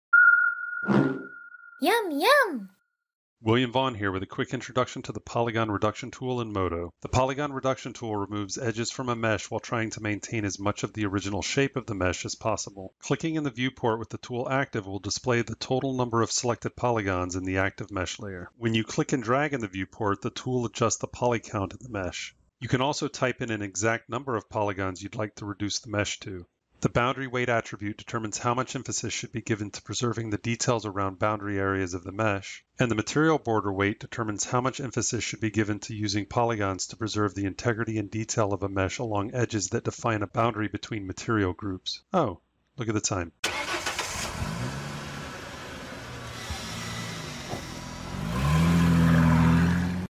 MODO | Polygon Reduction Tool
The Polygon Reduction tool removes edges from a mesh while trying to maintain as much of the original shape of the mesh as possible. This video is a quick introduction to this powerful tool in MODO.